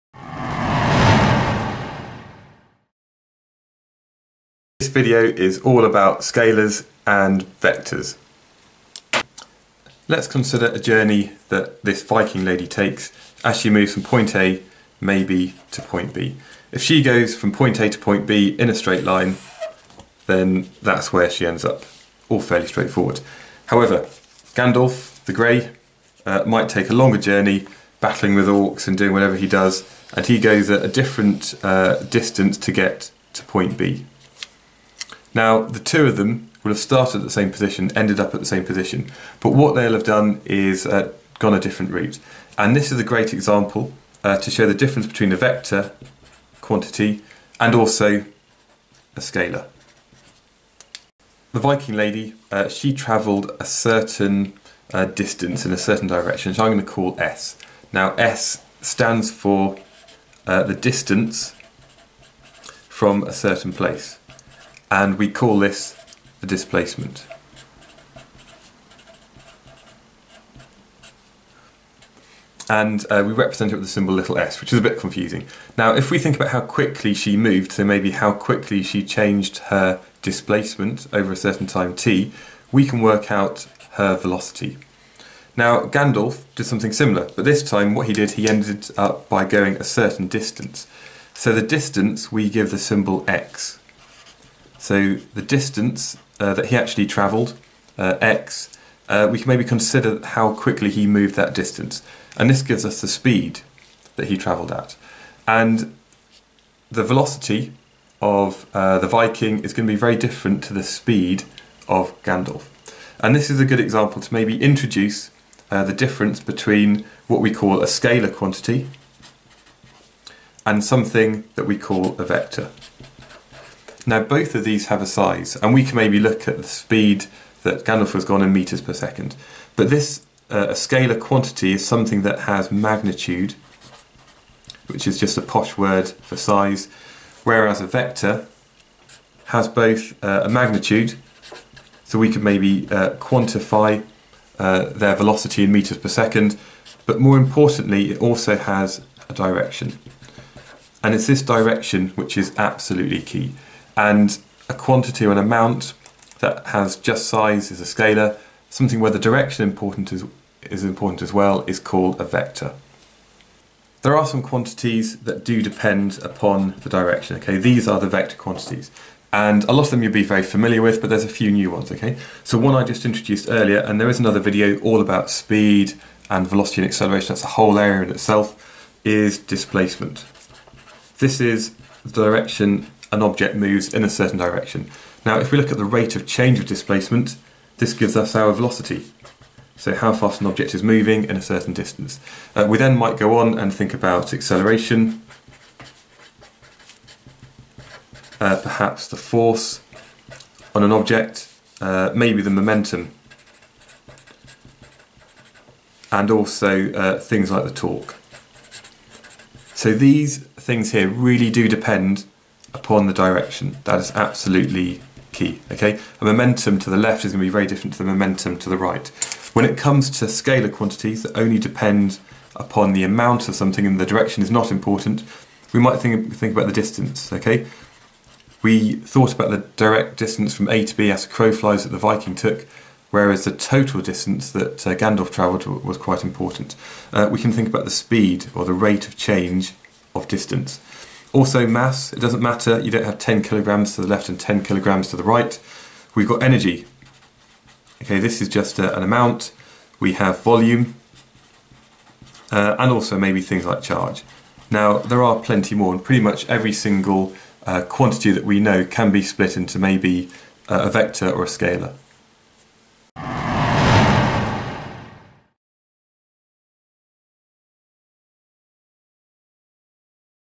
Scalars and Vectors - A Level Physics
This video introduces and explains scalars and vectors for A Level Physics.

A scalar quantity only has a size or magnitude i.e. they have no direction. Whereas a vector quantity has both a size, or magnitude, and a direction. Examples of scalar quantities are; speed, displacement and mass. Examples of vector quantities are; velocity, displacement, acceleration and weight (force).

Lewis

AQA
CIE
Edexcel
Edexcel IAL
Eduqas
IB
OCR A
OCR B
WJEC